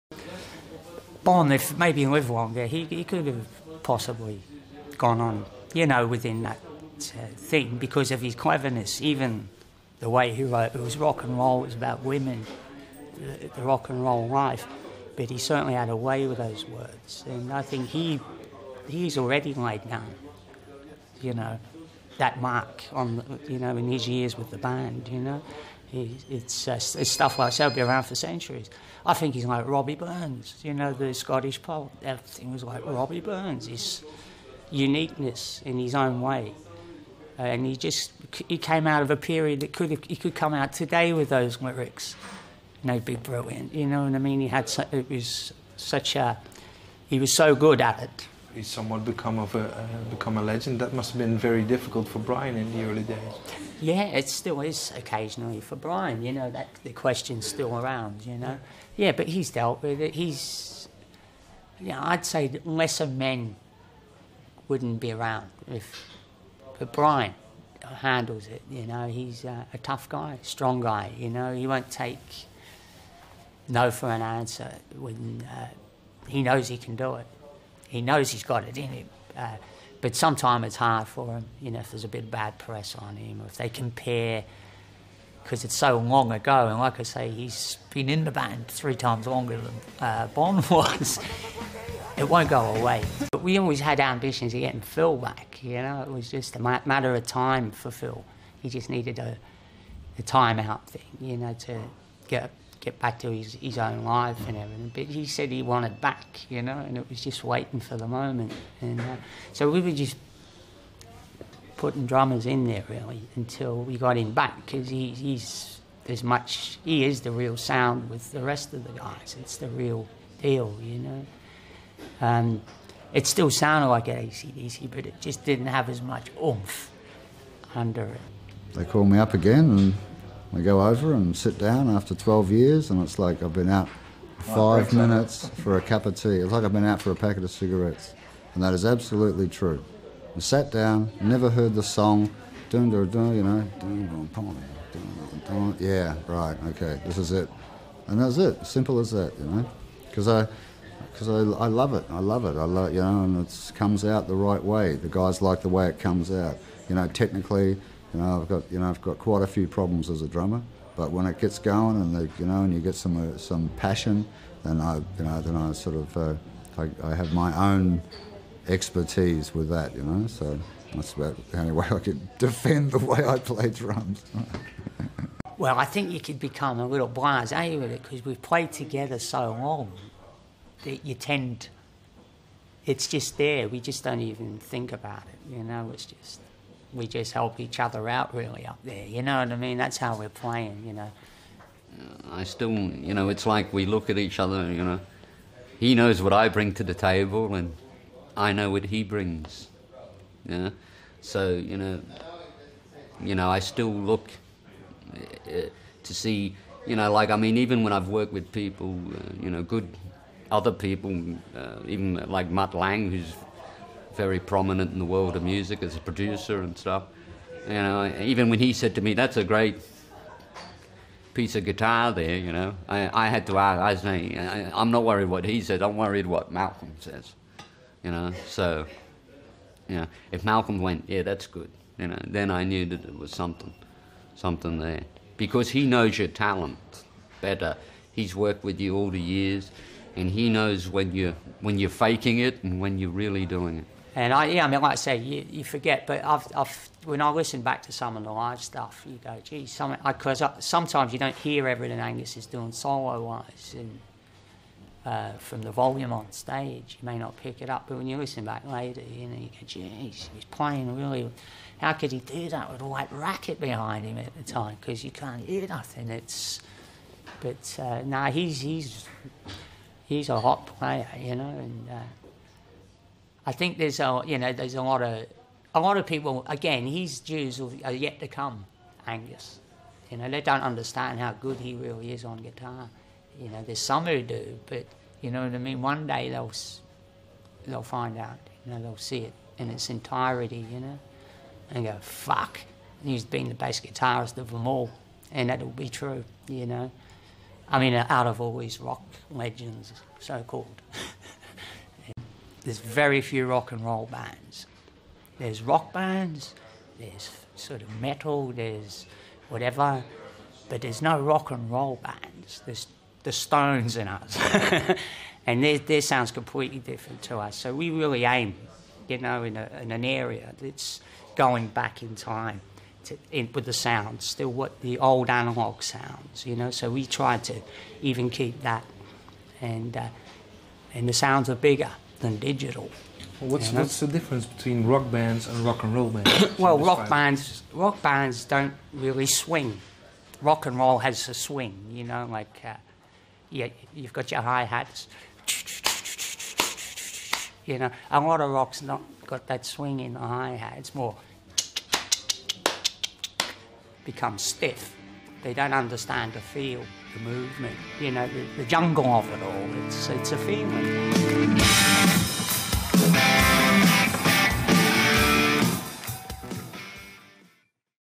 Malcolm Young Talks AC/DC, Angus Guitar Playing, Brian and Bon & Drummer Phil Rudd
The bloke is so normal its amazing